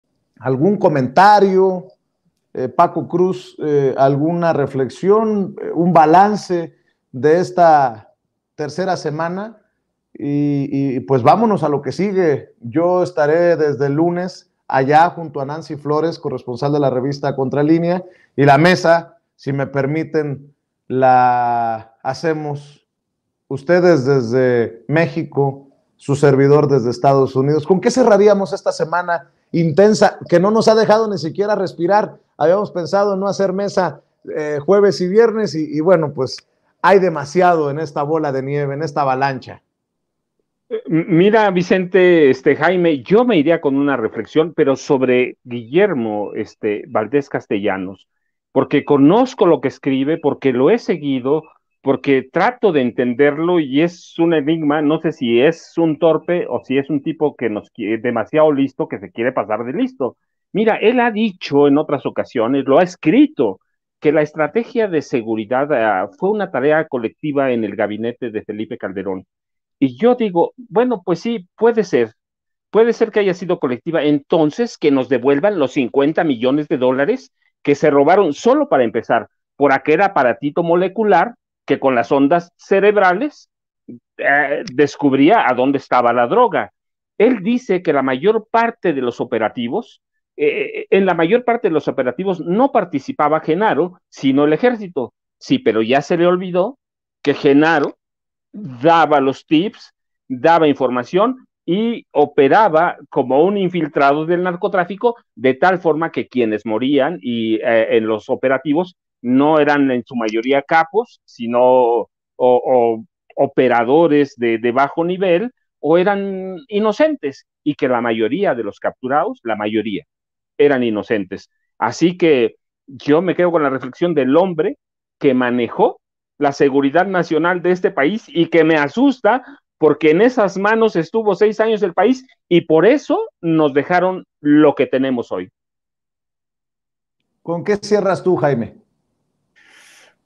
GUILLERMO VALDÉS SE QUIERE PASAR DE LISTO DEFENDIENDO A GARCÍA LUNA AFIRMA PACO CRUZ
El periodista y escritor Francisco Cruz hizo un balance del juicio contra Genaro García Luna, sobre todo ante la actitud del exdirector del Cisen, Guillermo Valdés. Paco considera que Valdés se quiere pasar de listo, ya que dice que en la mayor parte de los operativos no participaba el exsecretario de Seguridad Pública de Calderón, sino el Ejército, pero sí tenía acceso a esa información.